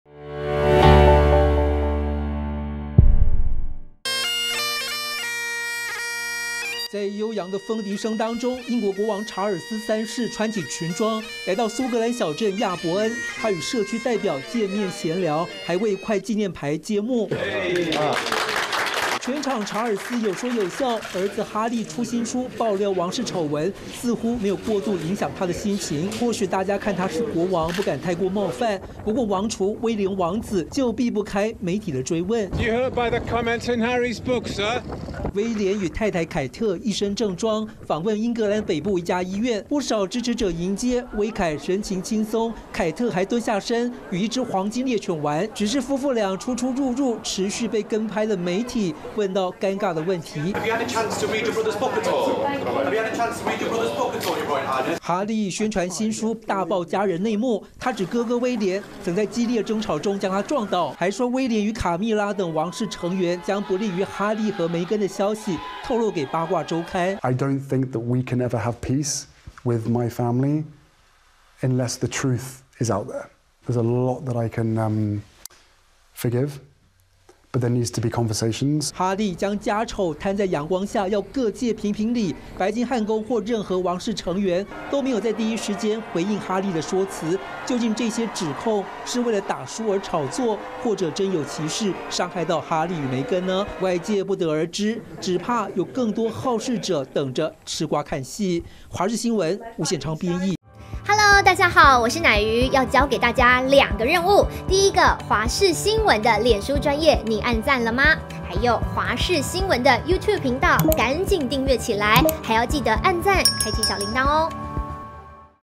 哈利出書後 查爾斯.威廉首度現身未置評｜華視新聞 20230113
英國哈利王子，最新出版的回憶錄，對許多王室成員做出不利指控。而他的父親查爾斯國王，以及哥哥威廉王儲，在12日公開露面時，並沒有對哈利的指控發表評論，很明顯在閃躲媒體的提問。王室低調的態度，和哈利高調搏版面的作法，形成鮮明對比。